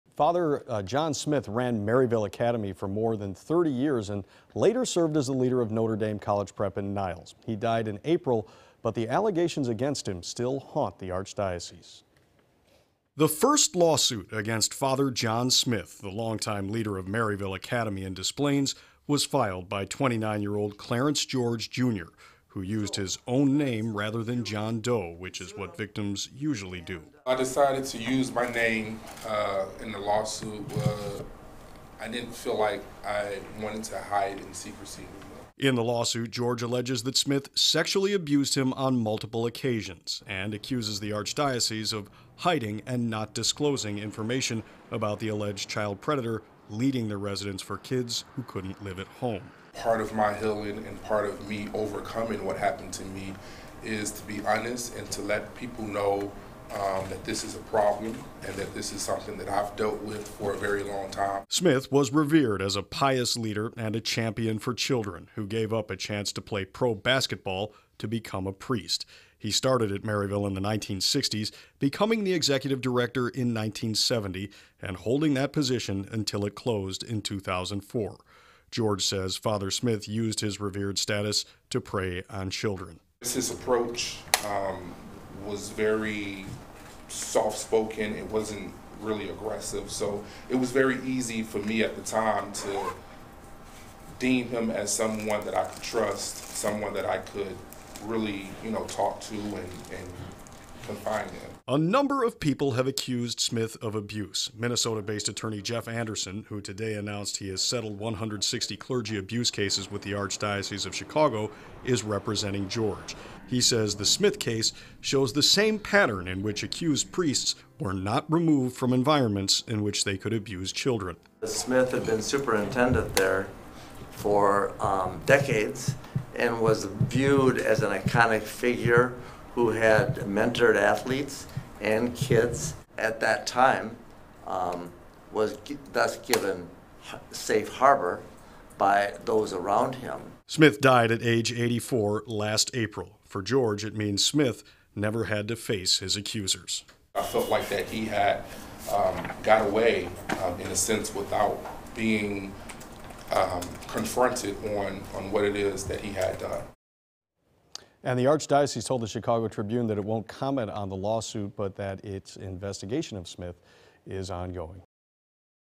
Lawsuit accuses former Maryville Academy head John Smyth of abuse
A prominent attorney filed lawsuit against the Archdiocese of Chicago Tuesday alleging Father John Smyth sexually abused a boy while he was in charge of Maryville Academy.